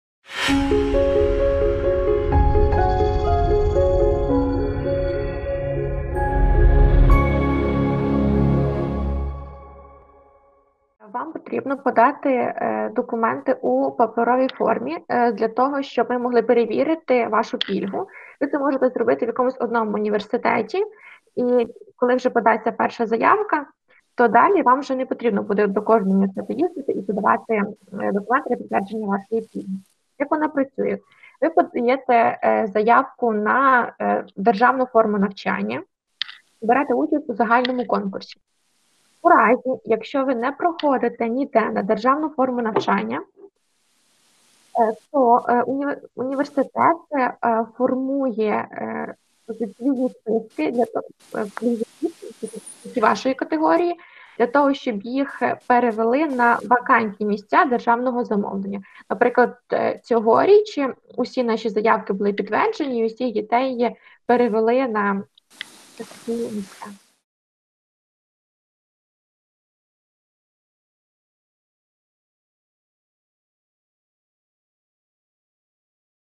Tips26 Вступ до Острозької академії: пільговий вступ для дітей учасників бойових дій
Контакти:
Приймальна комісія: (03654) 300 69,(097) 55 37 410